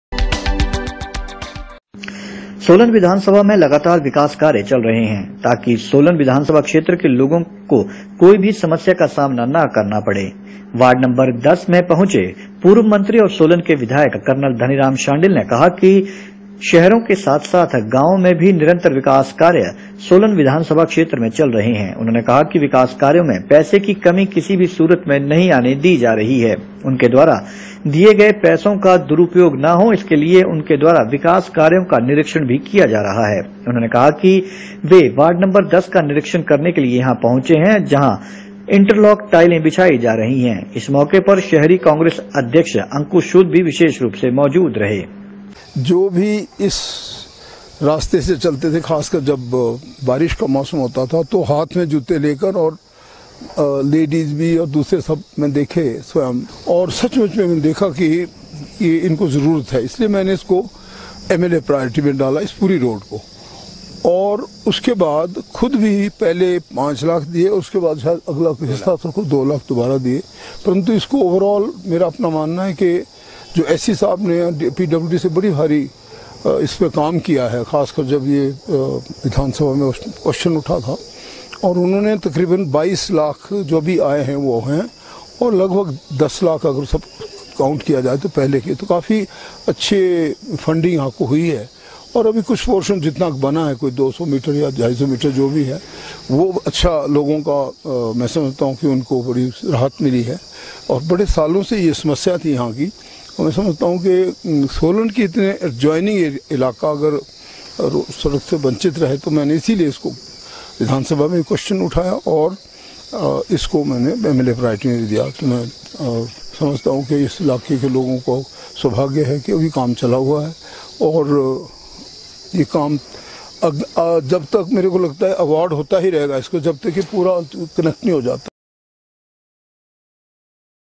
Solanवार्ड नं 10 में टाइलें लगाने के लिए धनीराम शांडिल ने दिए 22 लाख रुपये
सोलन विधानसभा में लगातार विकास कार्य चल रहे हैं ताकि सोलन विस के लोगों को कोई भी समस्या का सामना ना करना पड़े,आज वार्ड नंबर 10 में पहुंचे पूर्व मंत्री और सोलन के विधायक कर्नल धनीराम शांडिल ने कहा कि शहरों के साथ-साथ गांव में भी निरंतर विकास कार्य सोलन विधानसभा क्षेत्र में चल रहे हैं। उन्होंने कहा कि विकास कार्यों में पैसे की कमी किसी भी सूरत में नहीं आने दी जा रही है।   उनके द्वारा दिए गए पैसे का दुरुपयोग न हो इसके लिए  उनके द्वारा विकास कार्यों का निरीक्षण भी किया जा रहा है। उन्होंने कहा कि आज वह वार्ड 10 का निरीक्षण करने के लिए पहुंचे हैं।  जहां इंटरलॉक टाइलें बिछाई जा रही है। इस मौके पर शहरी कांग्रेस अध्यक्ष अंकुश सूद भी विशेष रूप से मौजूद रहे। @ Solan @MLA DhaniRam @ Solan MLA @ Congress